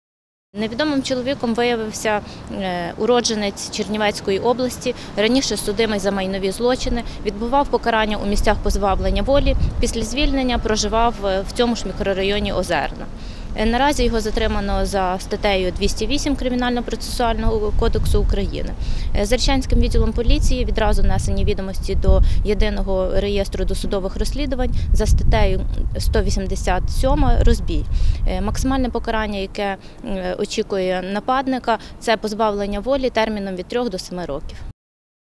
У Хмельницькому пограбували школярів
На території 28 школи в мікрорайоні Озерна в Хмельницькому затримали чоловіка, який пограбував двох школярів. Про це повідомила інспекторка сектору моніторингу Хмельницького відділу поліції, Людмила Берестовська.  За її словами,  невідомий,  погрожуючи ножем, відібрав у  дітей мобільні телефони. Грабіжника затримав вчитель.